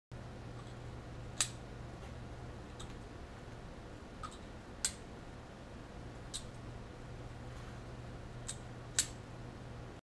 Magnetic Pen Demonstration
This is a demo of The Monarch Pen. These Handcrafted Fountain Pens feature a strong magnet inside the cap. This allows the cap to post smoothly on either end of the pen. Once you start playing with this pen and its unique lid you won't want to put it down!